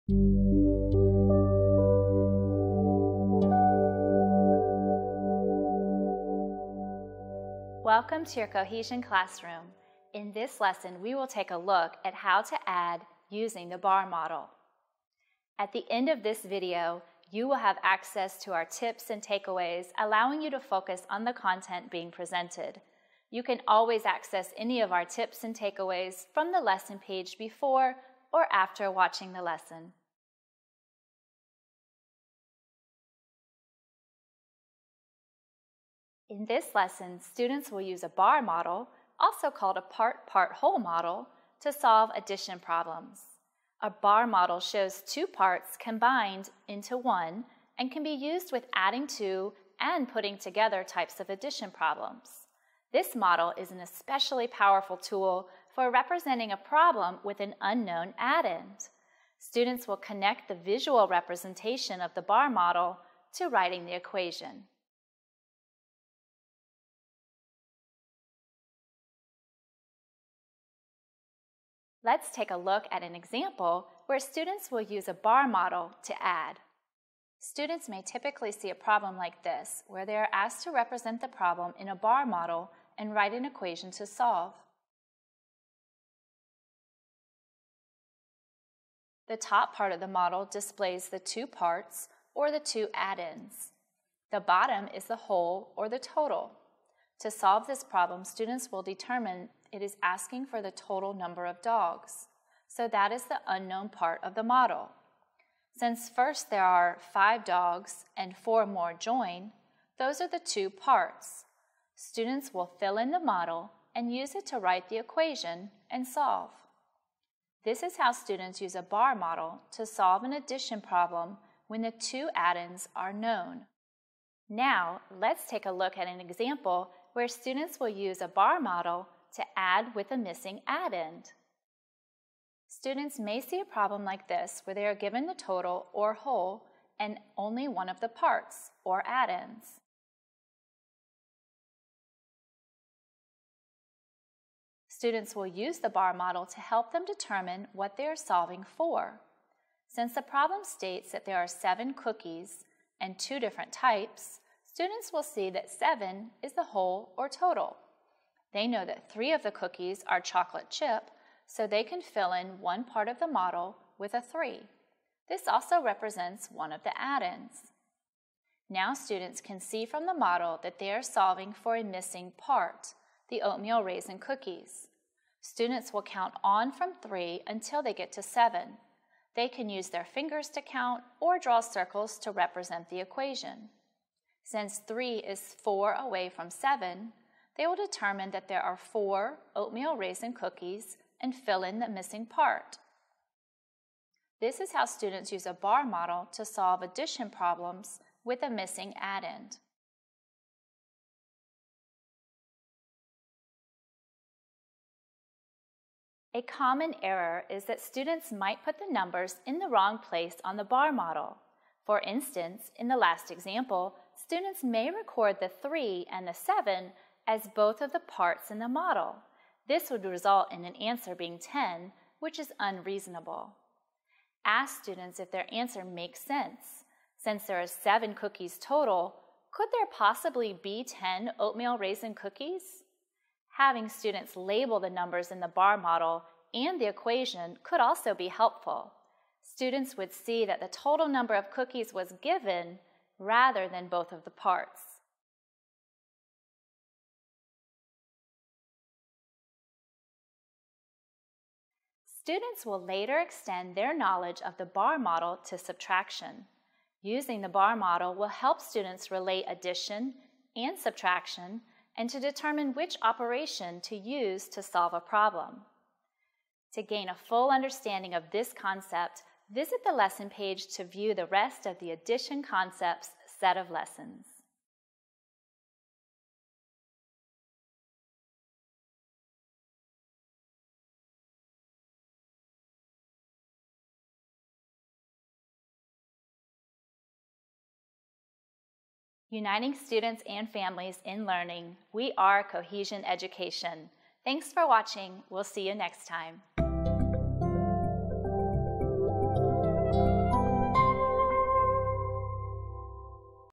Add Using the Bar Model
In this lesson students will use a bar model, also called a part-part-whole model, to solve addition problems. A bar model shows 2 parts combined into one and can be used with ‘adding to’ and ‘putting together’ types of addition problems. This model is an especially powerful tool for representing a problem with an unknown addend.